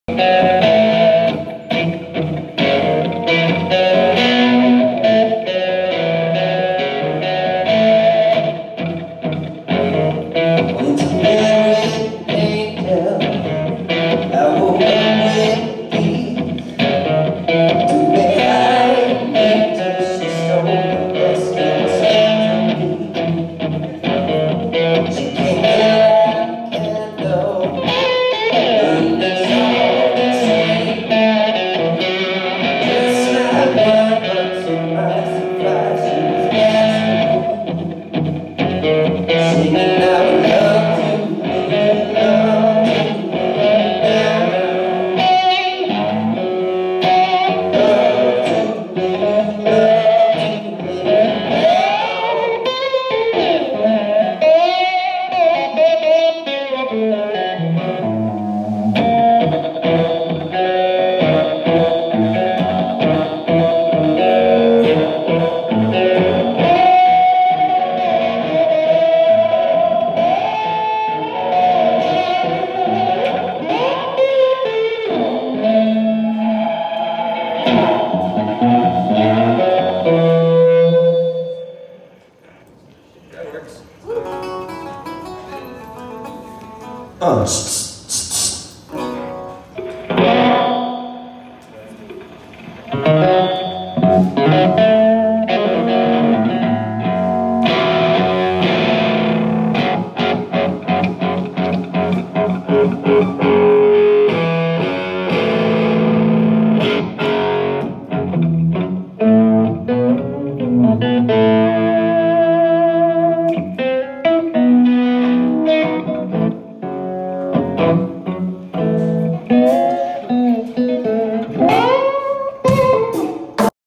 Tyler Bryant and the Shakedown
Tyler quick jam during vip soundcheck